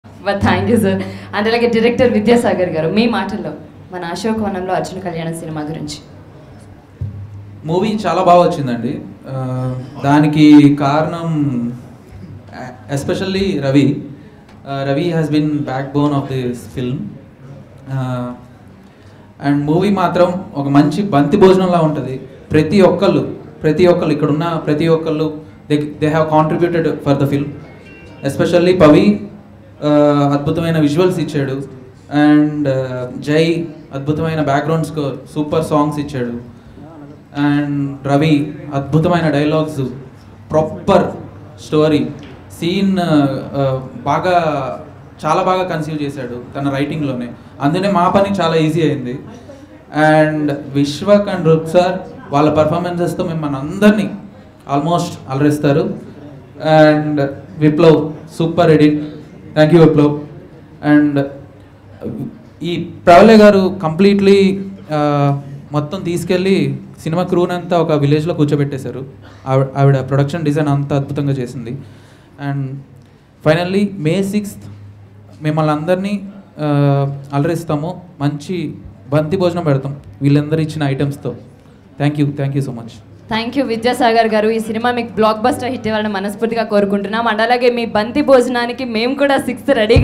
Director Vidya Sagar Chinta Speech @ Ashoka Vanamlo Arjuna Kalyanam Press Meet | Shreyas Media
Watch Director VidyaSagar Chinta Speech @ AshokaVanamloArjunaKalyanam Press Meet